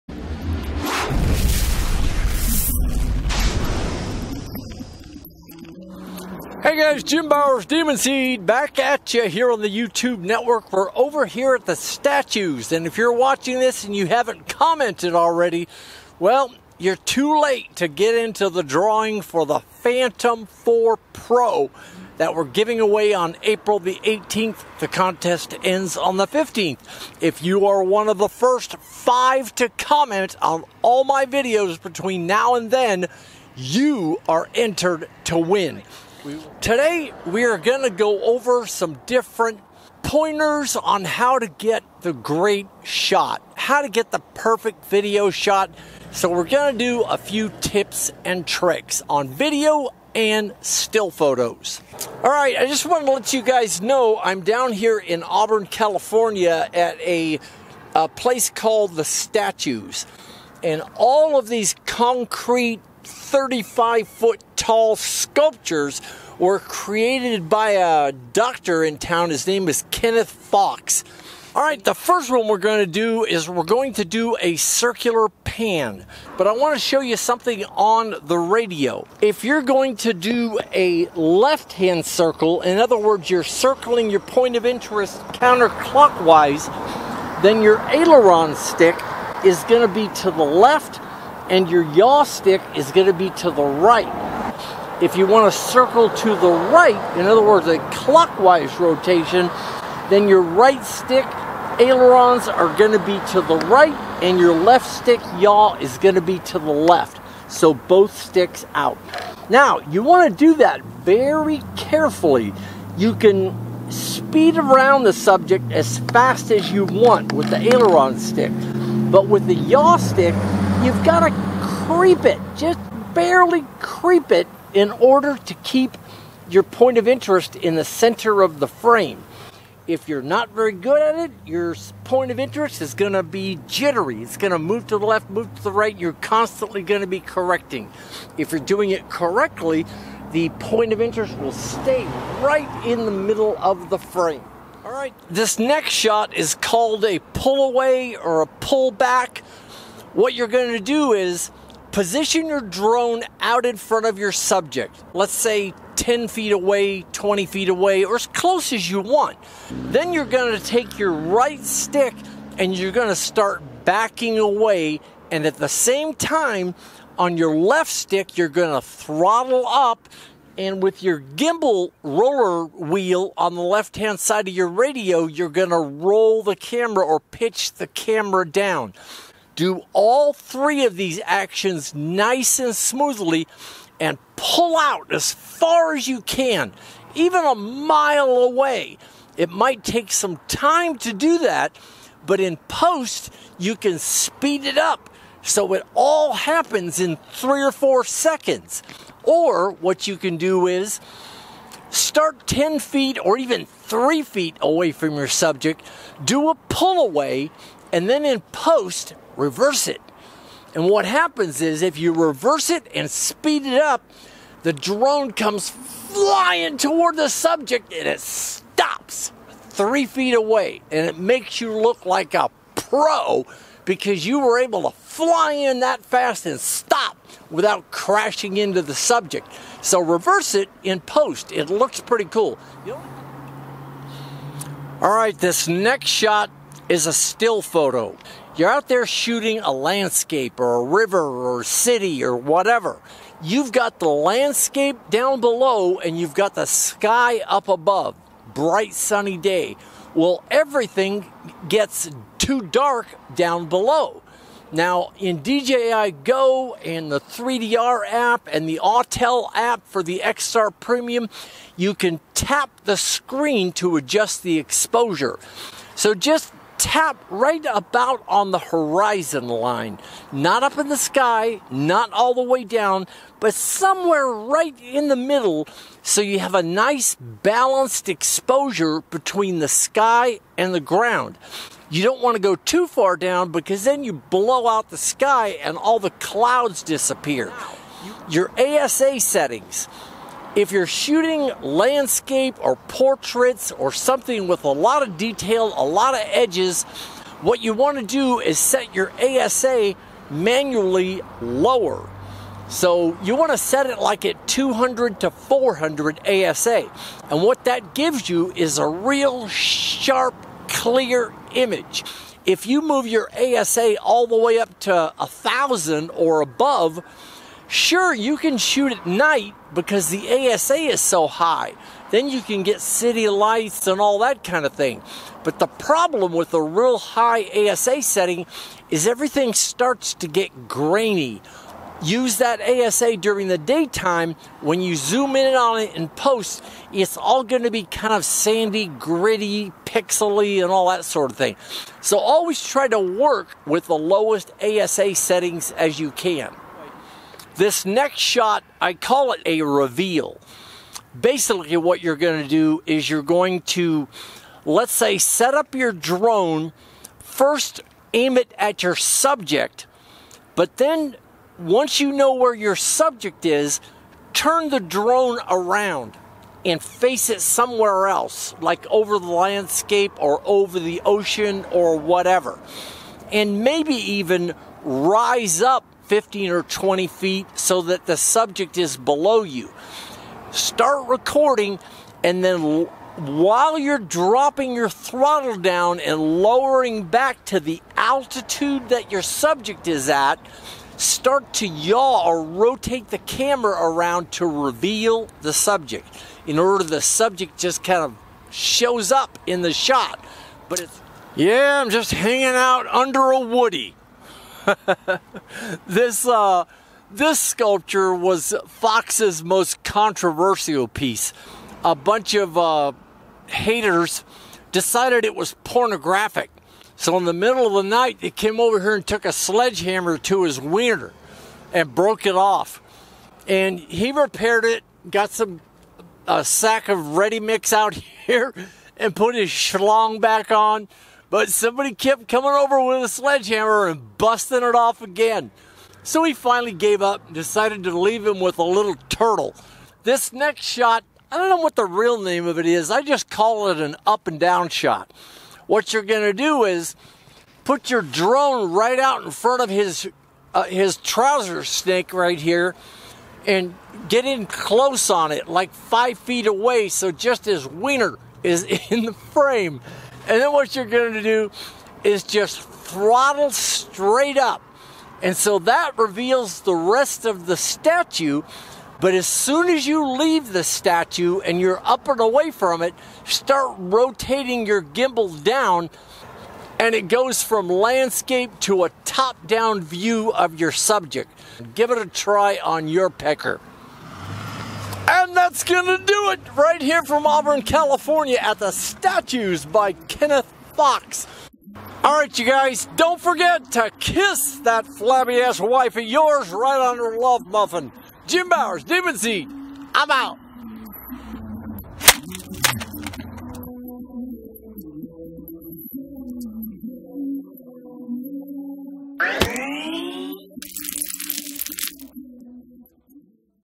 Drone Camera Tips for Dummies! - Demunseed
Here are a few tips and tricks for getting a great video or still shot with your drone.